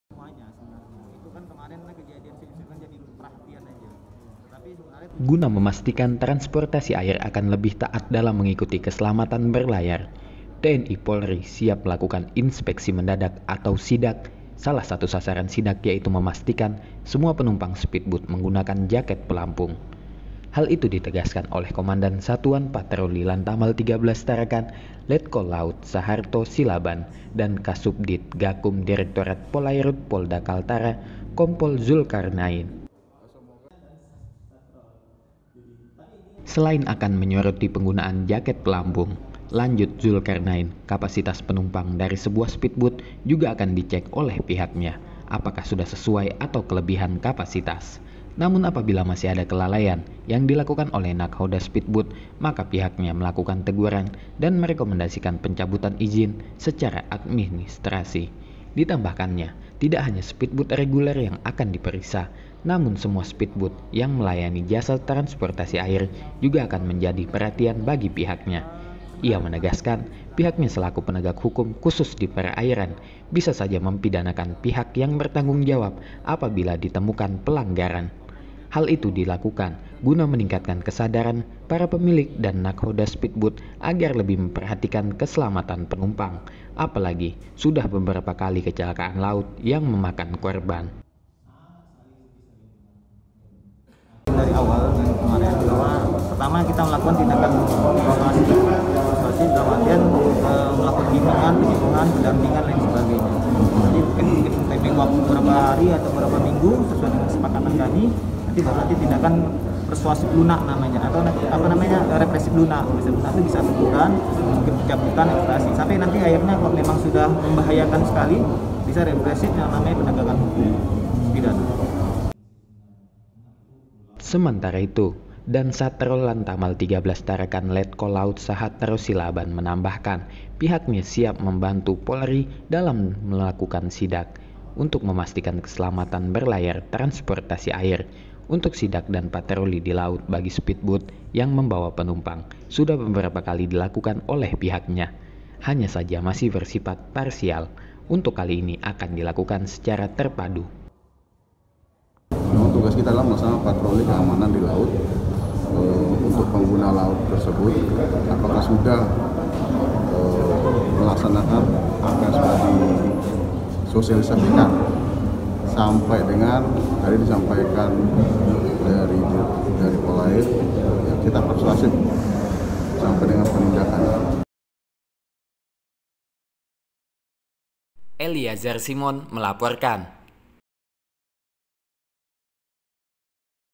TNI-Polri Siap Sidak Speedboat
Cek Keselamatan Berlayar

Untuk memastikan transportasi air akan lebih taat dalam mengikuti keselamatan berlayar, TNI-Polri siap melalukan inspeksi  mendadak (sidak). Salah satu sasaran sidak yaitu memastikan semua penumpang speedboat menggunakan jaket pelampung. Hal itu ditegaskan oleh Komandan Satuan Patroli (Dansatrol) Lantamal XIII Tarakan, Letkol Laut (P) Sahatro Silaban dan Kasubdit Gakkum Direktorat Polairud Polda Kaltara, Kompol Zulkarnain.
Reporter: Eliazar Simon